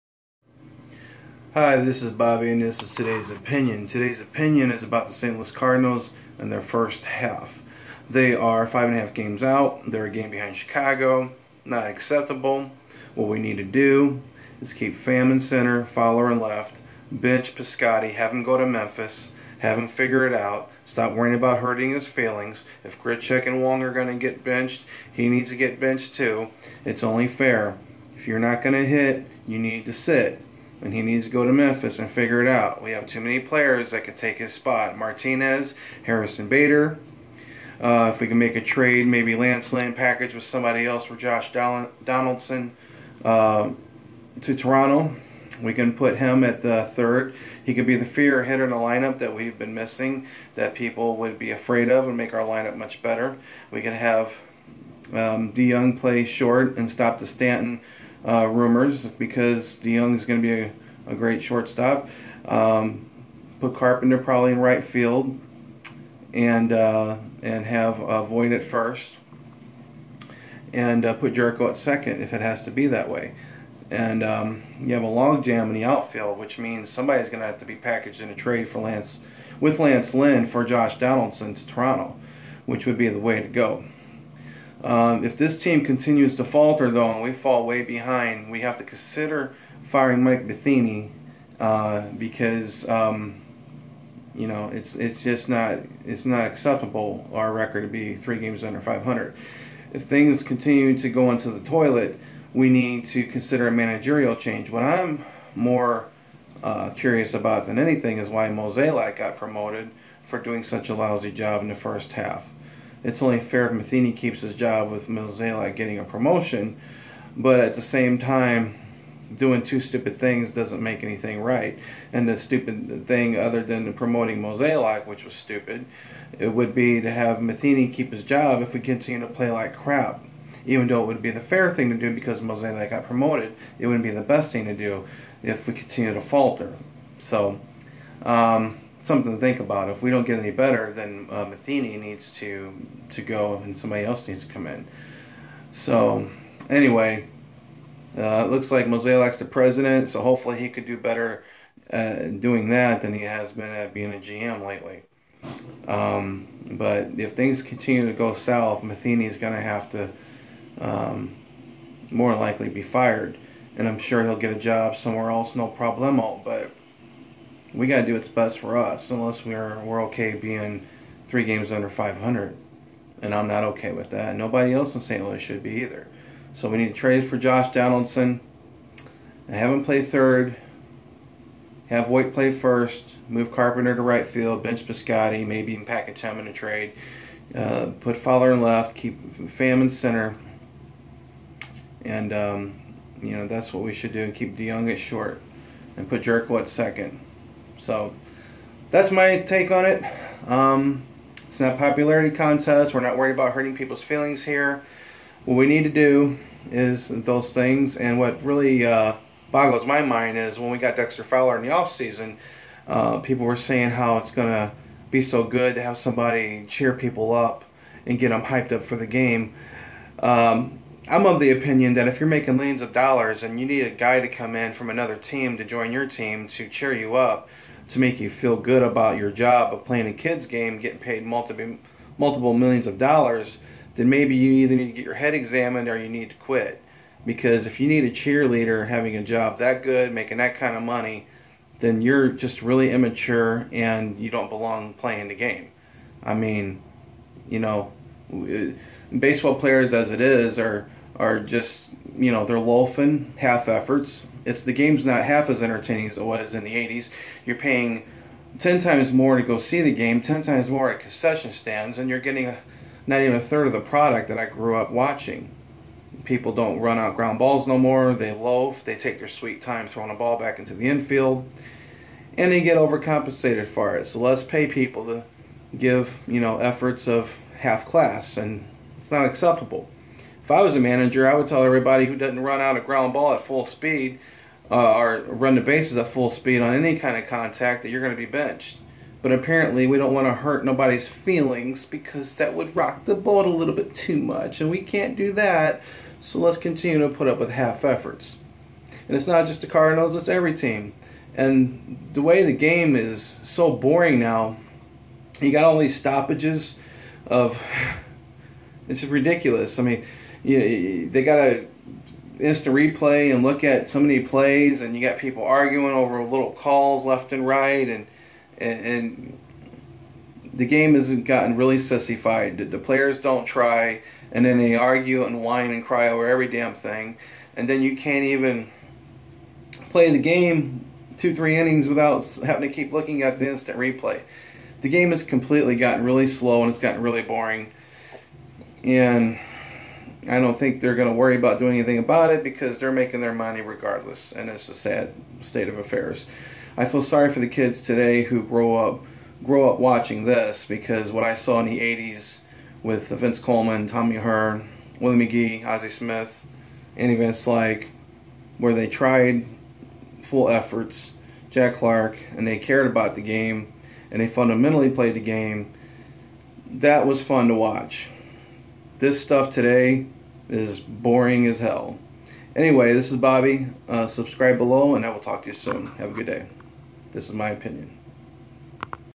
7/11/2017 cardinals dismal first half
Boring first half for the cardinals ...yawn...